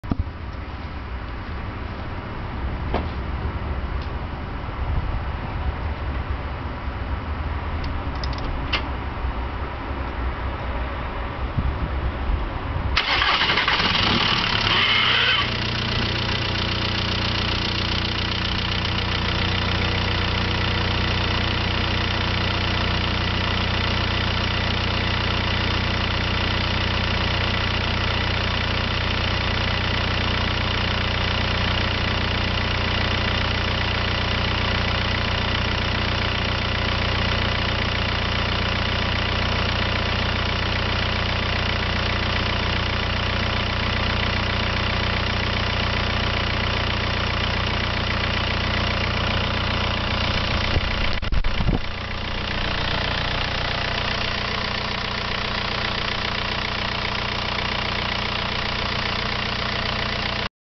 me starting my 2000 TDI New Beetle at 47º
that's turbo direct injection for ya'll that dont know what TDI Means. you can hear my starter stick a lil at start up! lol  i did replace it !!